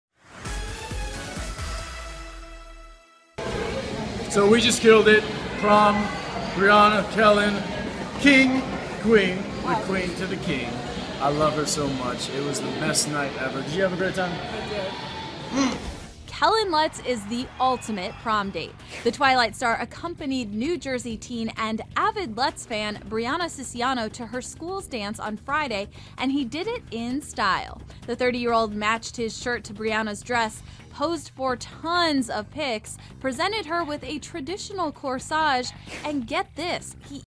Kellan Lutz Takes One Of His Biggest Fans to Prom
Don't ever say Kellan Lutz isn't good to his fans!